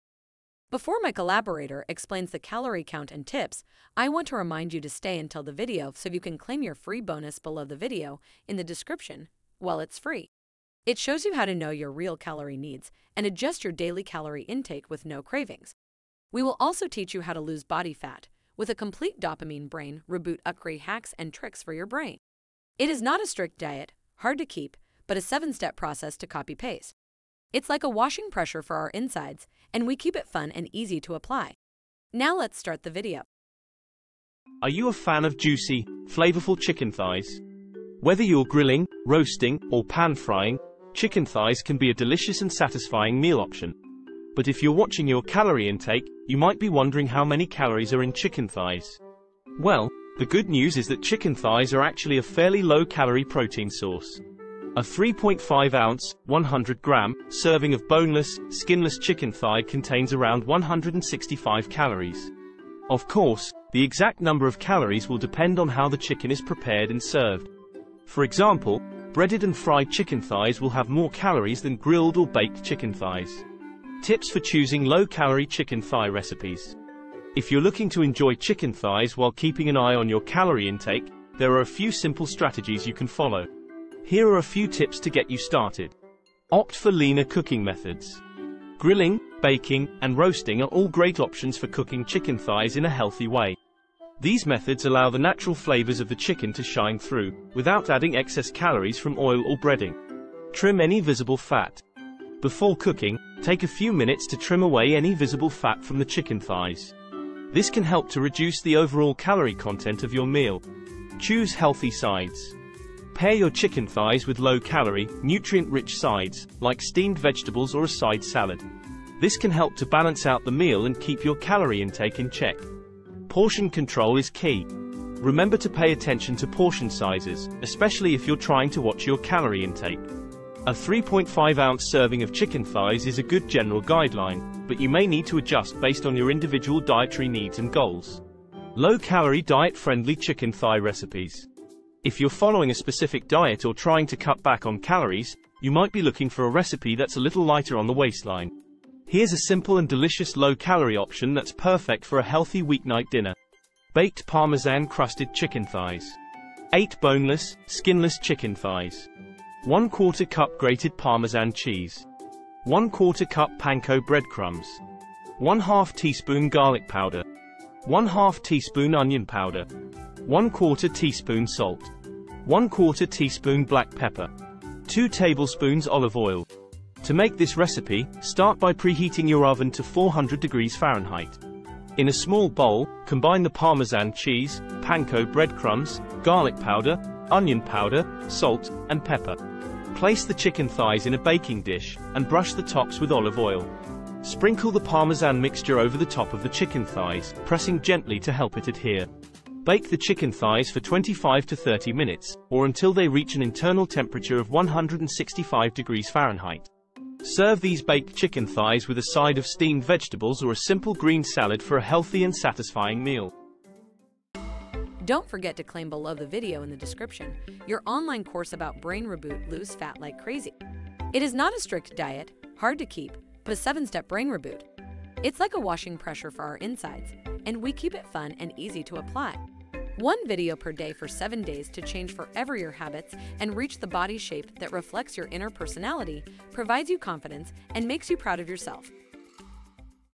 How Many Calories In Chicken Thigh And Low-Calorie Recipes
🍽️ Recipe & Calories:

Are you a fan of juicy, flavorful chicken thighs?

Whether you're grilling, roasting, or pan-frying, chicken thighs can be a delicious and satisfying meal option. But if you're watching your calorie intake, you might be wondering how many calories are in chicken thighs.

Well, the good news is that chicken thighs are actually a fairly low-calorie protein source!

A 3.5-ounce (100-gram) serving of boneless, skinless chicken thigh contains around 165 calories.

Of course, the exact number of calories will depend on how the chicken is prepared and served.

For example, breaded and fried chicken thighs will have more calories than grilled or baked chicken thighs.

How Do Chicken Thigh Calories Compare to Other Types of Chicken?

When it comes to chicken, there are a few different cuts you can choose from.

Some common options include breasts, wings, and legs.

So, how do chicken thigh calories compare to these other cuts?

Generally speaking, chicken breasts tend to be the leanest option, with a 3.5-ounce serving containing around 140 calories/

On the other hand, chicken wings are a bit higher in calories, with a serving of about 170 calories.

And chicken legs tend to fall somewhere in the middle, with a 3.5-ounce serving containing around 160 calories.

So, as you can see, chicken thighs are a little higher in calories than breasts, but still lower than wings.

However, it's important to note that the calorie content can vary depending on the specific cut and how it's prepared.

For example, a bone-in, skin-on chicken thigh will have more calories than a boneless, skinless chicken thigh.

Tips for Choosing Low-Calorie Chicken Thigh Recipes
If you're looking to enjoy chicken thighs while keeping an eye on your calorie intake, there are a few simple strategies you can follow. Here are a few tips to get you started:

Opt for leaner cooking methods. Grilling, baking, and roasting are all great options for cooking chicken thighs in a healthy way. These methods allow the natural flavors of the chicken to shine through, without adding excess calories from oil or breading.

Low-Calorie Diet-Friendly Chicken Thigh Recipes

Now that you know all about chicken thigh calories, it's time to get cooking! Here are a few tasty and healthy chicken thigh recipes to try:

If you're following a specific diet or trying to cut back on calories, you might be looking for a recipe that's a little lighter on the waistline. Here's a simple and delicious low-calorie option that's perfect for a healthy weeknight dinner:

Baked Parmesan Crusted Chicken Thighs
8 boneless, skinless chicken thighs
1/4 cup grated Parmesan cheese
1/4 cup panko breadcrumbs
1/2 teaspoon garlic powder
1/2 teaspoon onion powder
1/4 teaspoon salt
1/4 teaspoon black pepper
2 tablespoons olive oil

To make this recipe, start by preheating your oven to 400°F. In a small bowl, combine the Parmesan cheese, panko breadcrumbs, garlic powder, onion powder, salt, and pepper.

Place the chicken thighs in a baking dish, and brush the tops with olive oil.

Sprinkle the Parmesan mixture over the top of the chicken thighs, pressing gently to help it adhere. Bake the chicken thighs for 25-30 minutes, or until they reach an internal temperature of 165°F.

Serve these baked chicken thighs with a side of steamed vegetables or a simple green salad for a healthy and satisfying meal.